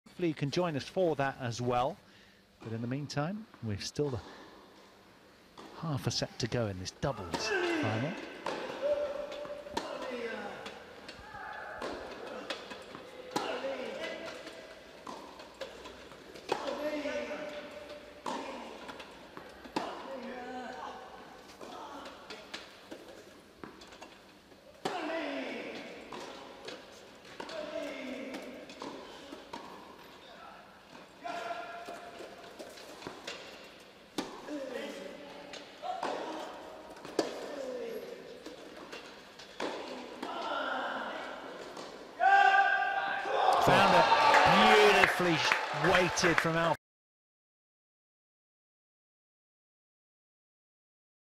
Incredible Alfie Hewett lob at the British Open
Alfie Hewett hits a brilliant lob during the final of the men's doubles at the British Open as he and Gordon Reid battled to defeat against Gustavo Fernandez & Shingo Kunieda in Nottingham.